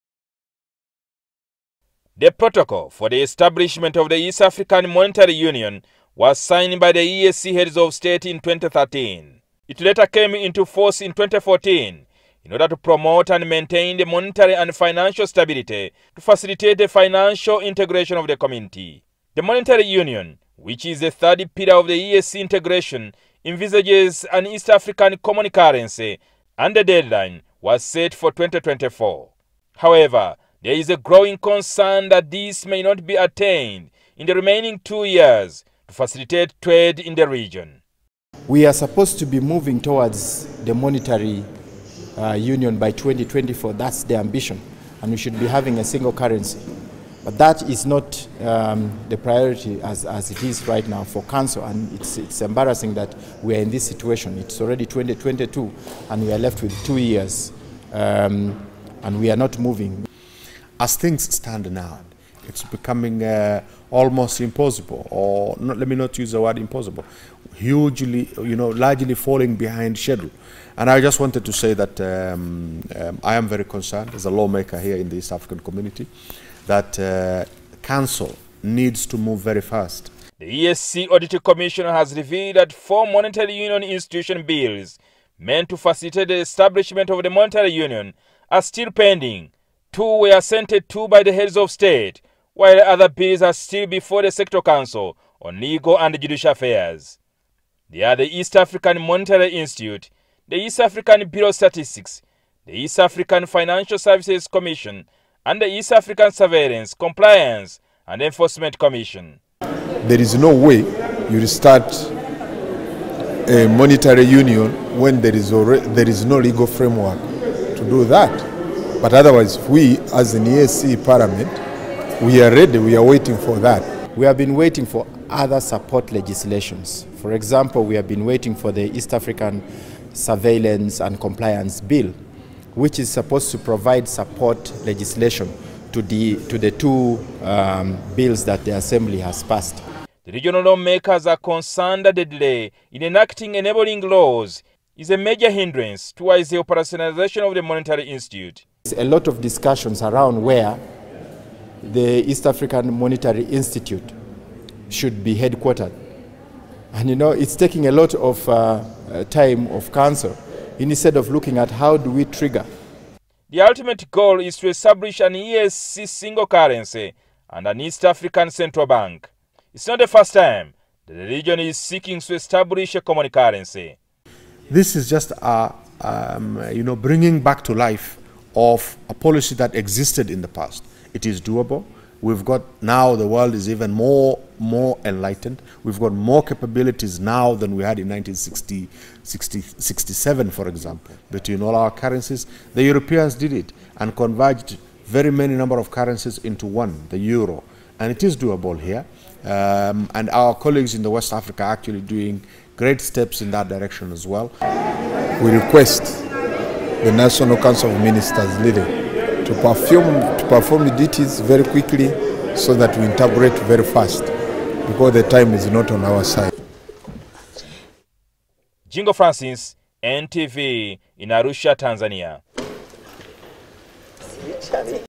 EAC set to miss 2024 deadline for monetary union implementation
The East African community could miss out on the 2024 deadline set for the implementation of the monetary union. The East African legislative assembly parliamentary committee on accounts is concerned that the delay is due to lengthy processes and decision making by the council of ministers. The monetary union envisages the establishment of a single currency and an East African central bank.  Jjingo Francis has details from Arusha Tanzania.